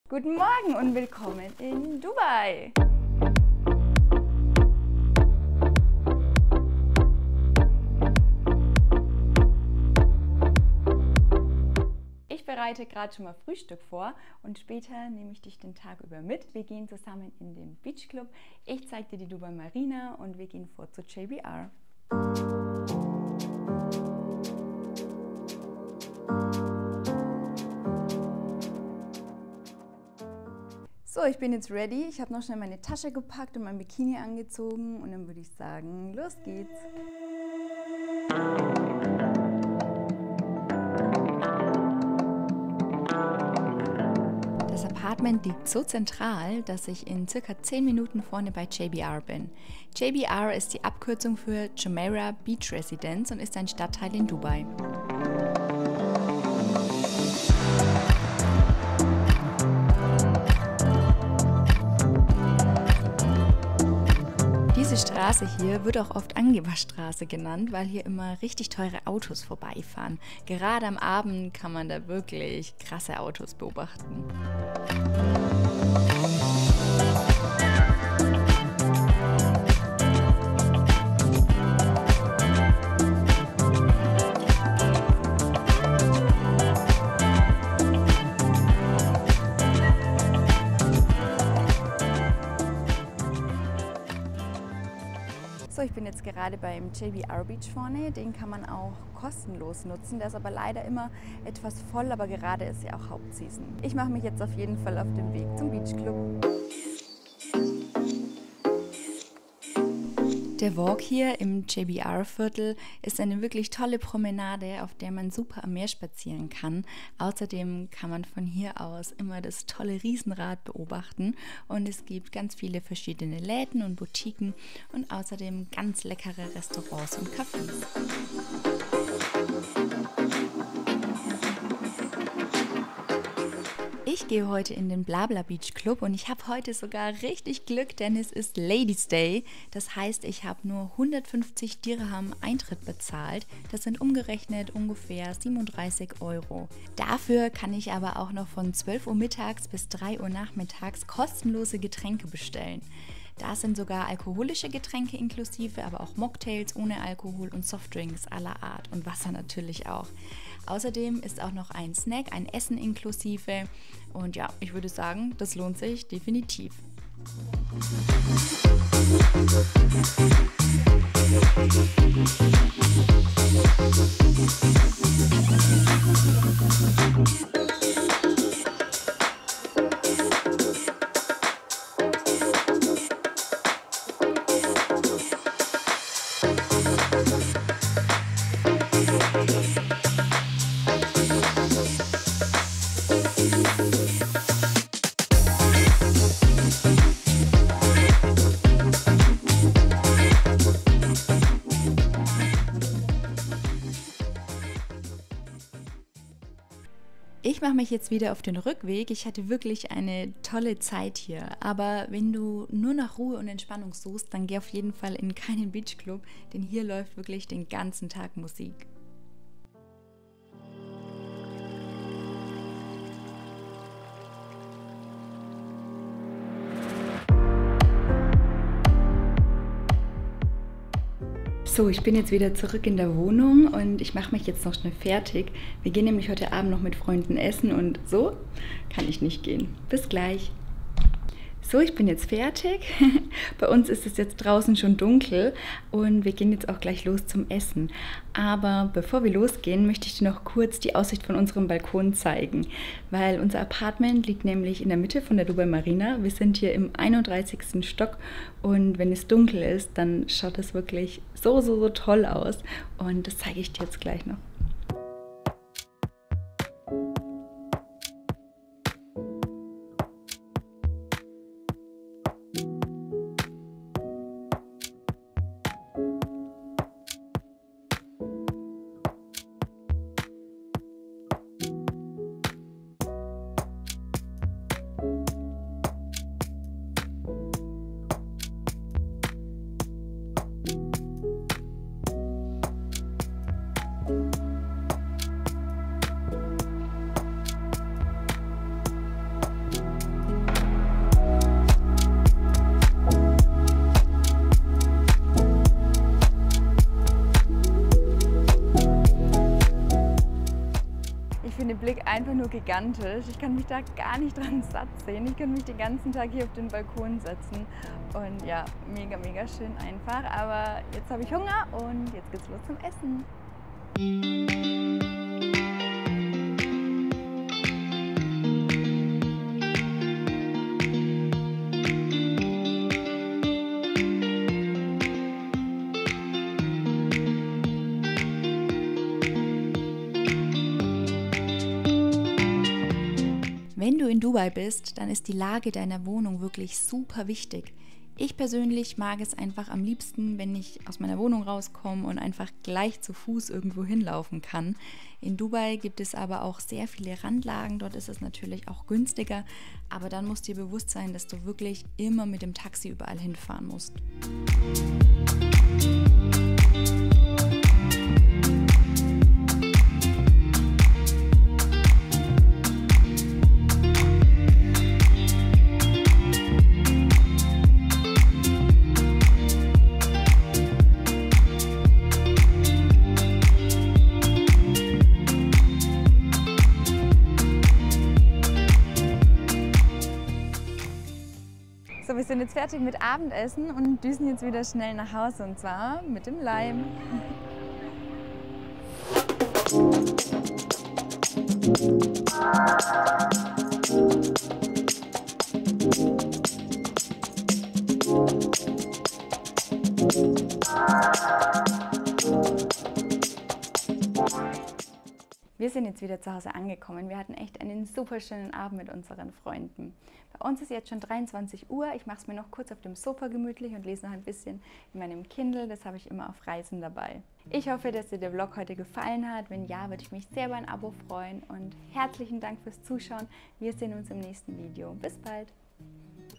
Dubai Vlog | JBR, Dubai Marina, Beach Club
In diesem Video nehme ich dich einen Tag lang mit und ich zeige dir in Dubai den Stadtteil JBR, die Dubai Marina und wir gehen zusammen in einen Beach Club. 😊

Begleite mich gerne, wenn dich interessiert, wie es in Dubai aussieht und was man dort unternehmen kann.

Aber jetzt erstmal viel Spaß beim Video. 😊

Deine Janine